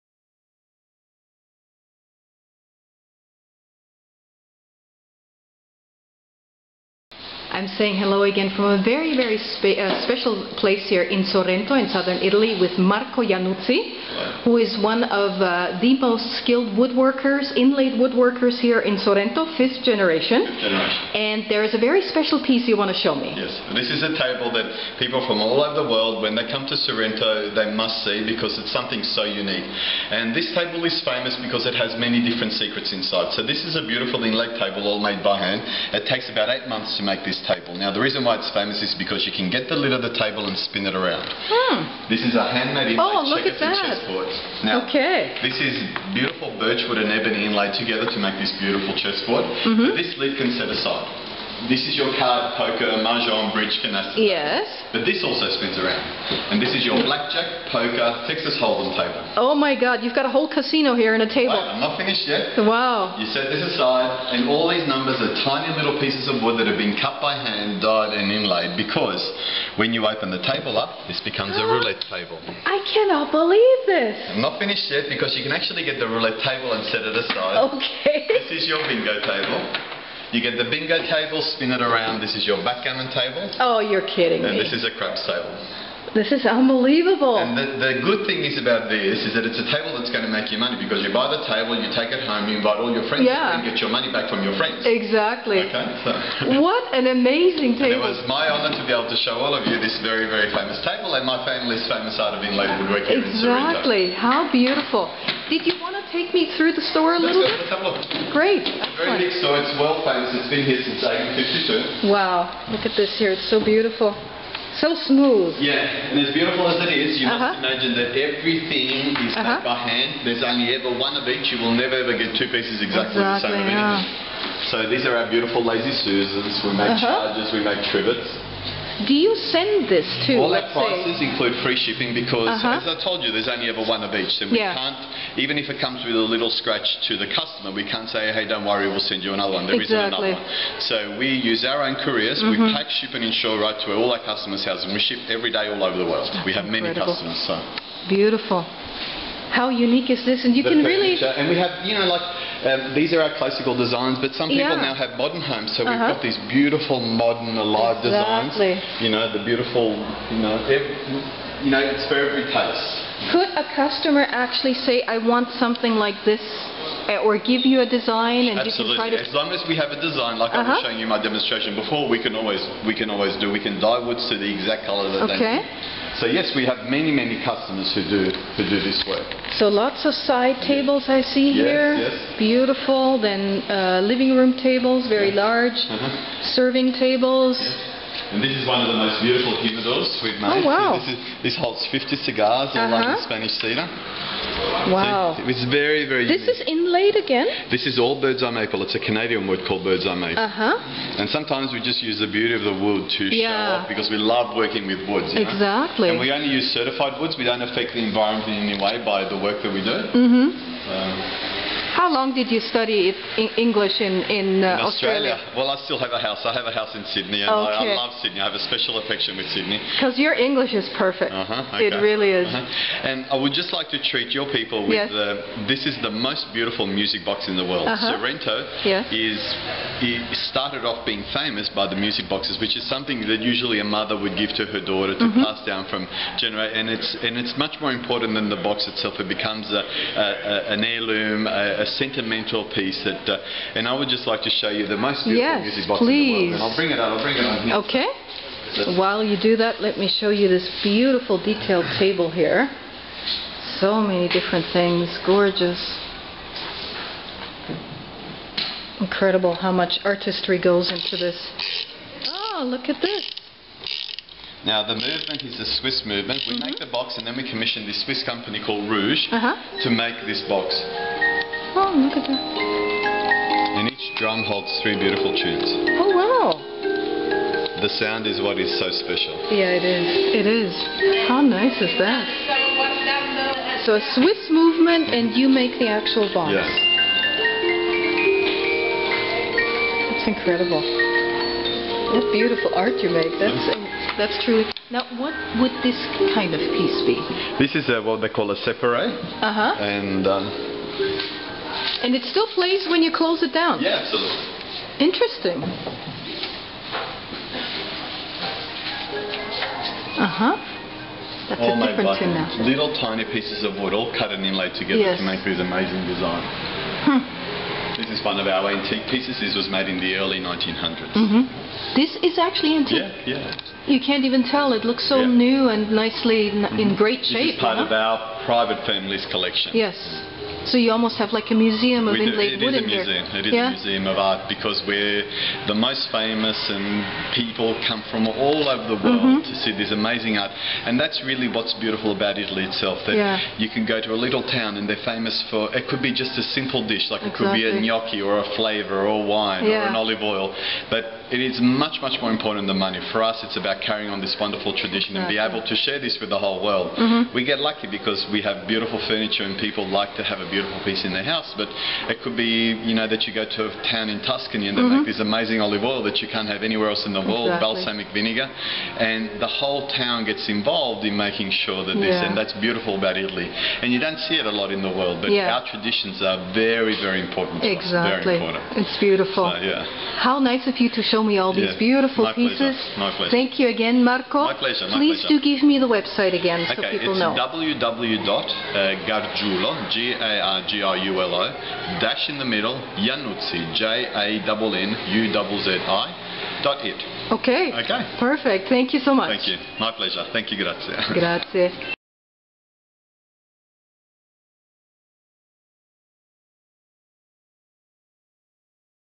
Travel Italy: 5th generation woodworking store in Sorrento
From April 20 to April 29, 2013, I spent 10 exciting days in Southern Italy. I started with two days in the historic city of Salerno, from where I also did an excursion to Amalfi and Ravello. From there I continued to Sorrento where I spent 6 days exploring local attractions, which included historic sites in town, two agriturismo operations, an electric bicycle tour as well as an entertaining performance of the "Sorrento Musical". During my time in Sorrento I also visited Positano, the beautiful island of Capri as well as towns on the Sorrentine Peninsula such as Massa Lubrenese and Sant'Agata. I spent my last two days in Italy in Naples where I did an extended city walking tour as well as a guided tour of the recently discovered Tunnel Borbonico on the first day. On the second day I went on a driving tour of the coastline northwest of Naples which included Marechiaro, Bagnoli, the "Campi Flegrei" (Phlegrean Fields) with the volcanic park of Solfatara, and continued to the villages of Baia and Miseno. During my last afternoon in Naples I explored the historic town of Caserta and its royal castle.
For full-length articles about these experiences please and [section about Naples still to be created].